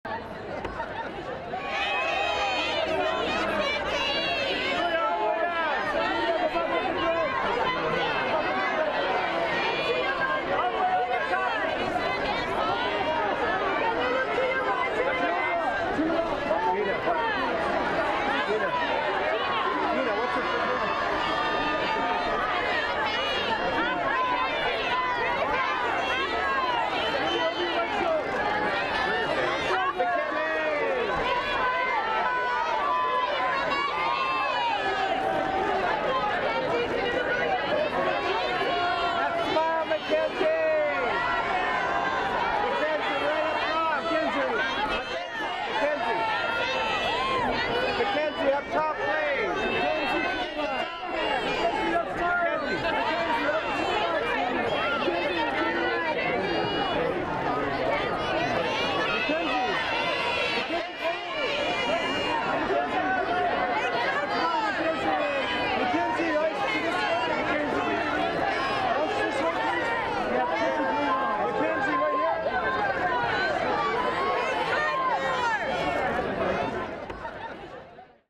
Mackenzie Foy TWILIGHT Breaking Dawn Part 2 Premiere ARRIVALS
Mackenzie Foy The Twilight Saga: BREAKING DAWN Part 2 Premiere Black Carpet Arrivals at Nokia Theater in Los Angeles, Ca USA November 12, 2012

©Ricomix Productions -